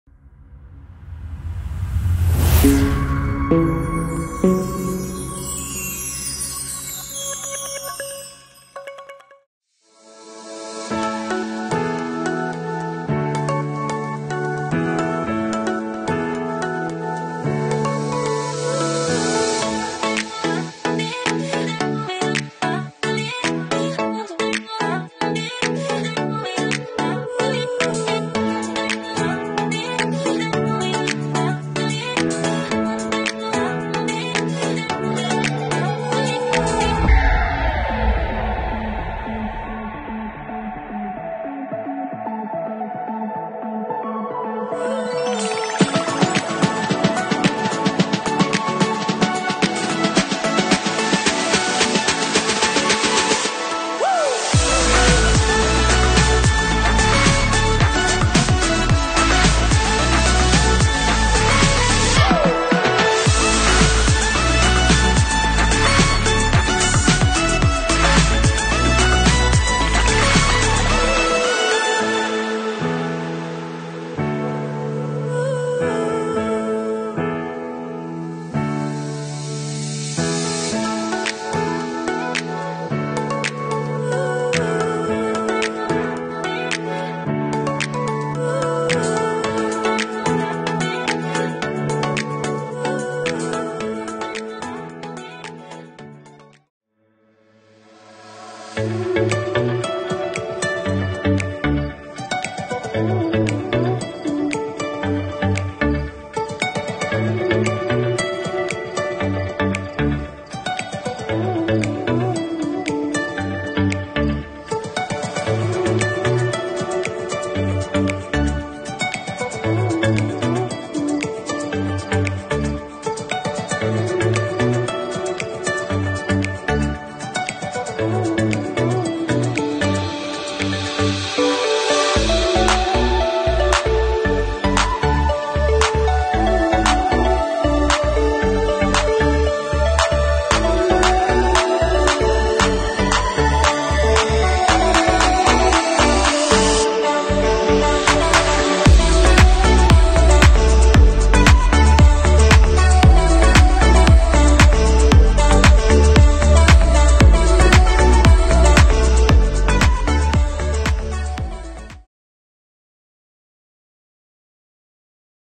Makeover Living & Dining Hall di Taman Bukit Dahlia, Pasir Gudang.
Jom tengok hasil team kami makeover ruang tamu dan ruang makan rumah customer kami di Taman Bukit Dahlia, Pasir Gudang.